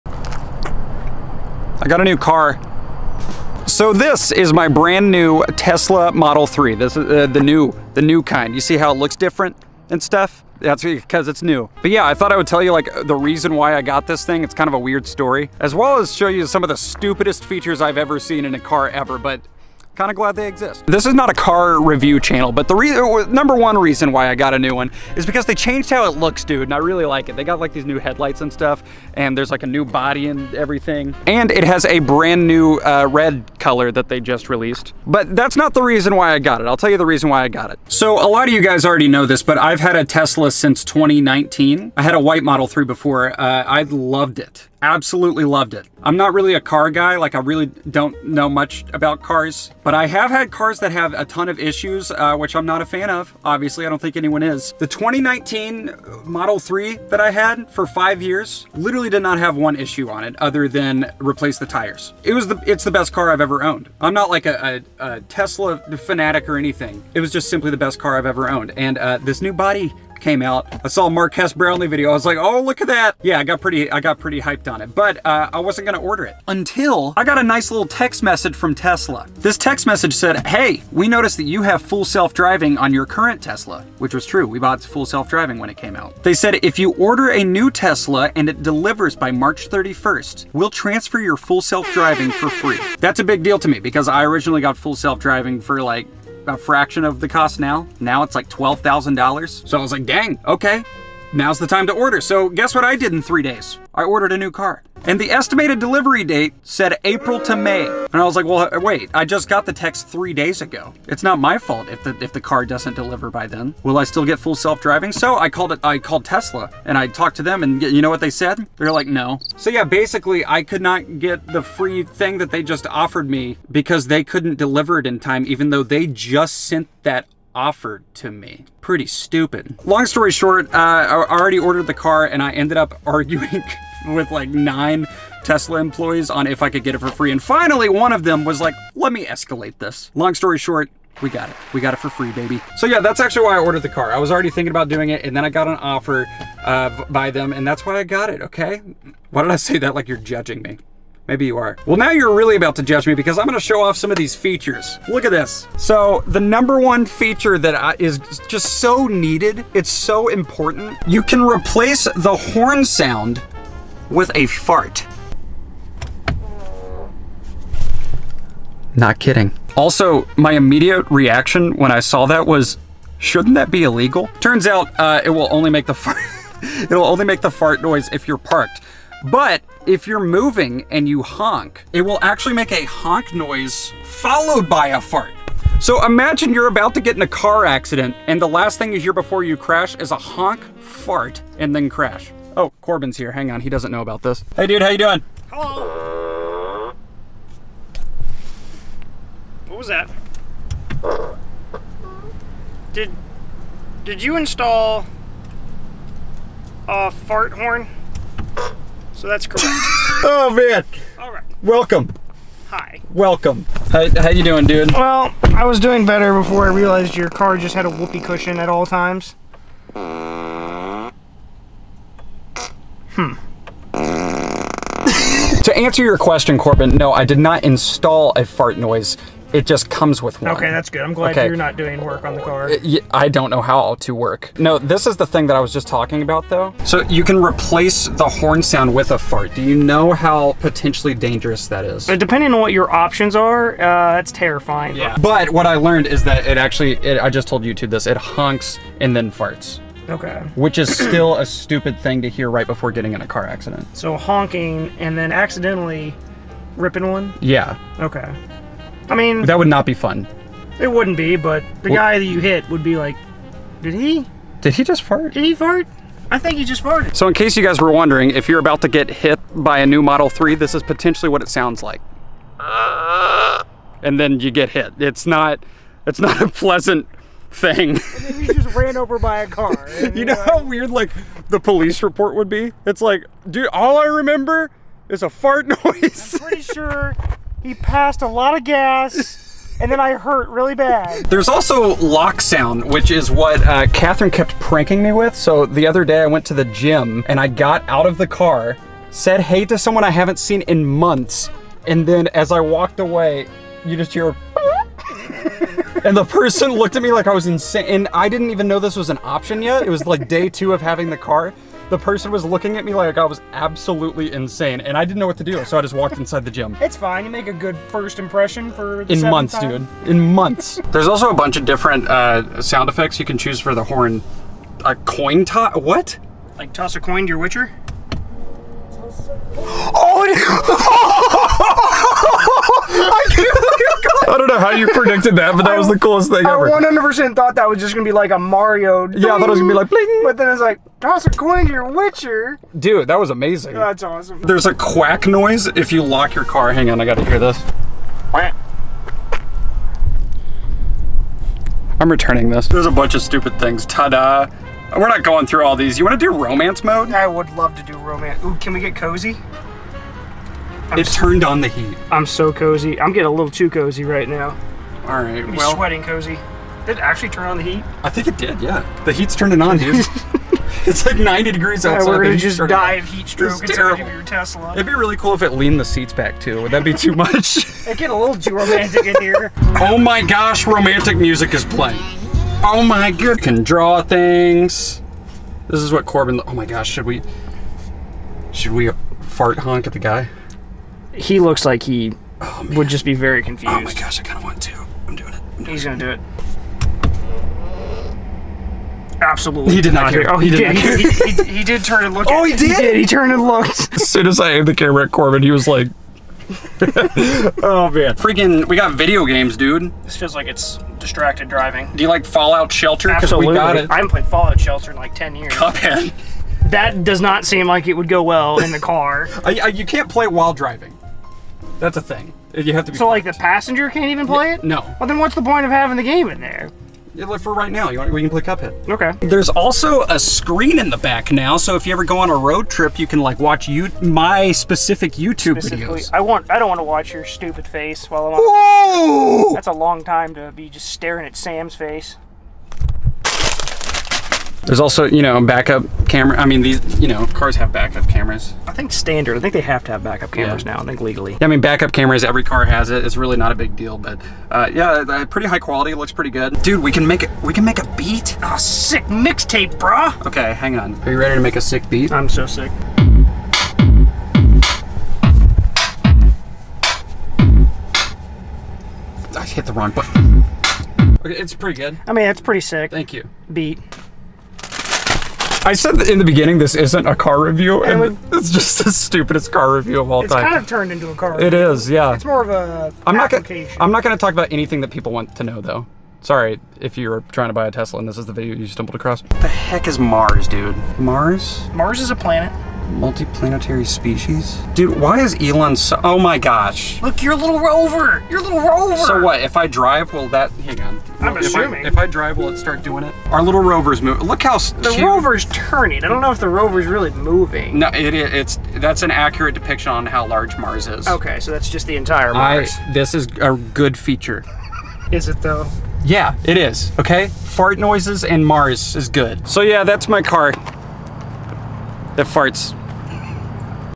My New Car Has a Fart Horn
I bought a new Tesla, and it came with a fart horn.